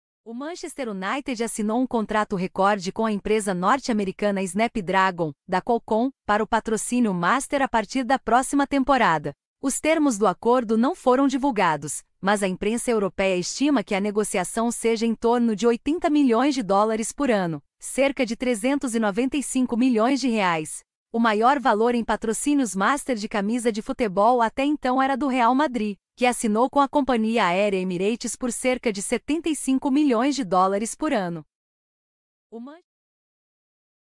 Manchester United fecha patrocínio recorde de R$ 395 milhões Por Patrocínio em Camisa
O Manchester United assinou um contrato recorde com a empresa norte-americana Snapdragon, da Qualcomm, para o patrocínio master a partir da próxima temporada. Os termos do acordo não foram divulgados, mas a imprensa europeia estima que a negociação seja em torno de US$ 80 milhões por ano (cerca de R$ 395 milhões). O maior valor em patrocínios master de camisa de futebol até então era do Real Madrid, que assinou com a companhia aérea Emirates por cerca de US$ 75 milhões por ano.
Nova camisa do Manchester United para a temporada 2023/24 — Foto: Divulgação
Varane, Sancho e Rashford comemoram vitória do Manchester United sobre o Wolverhampton — Foto: Reuters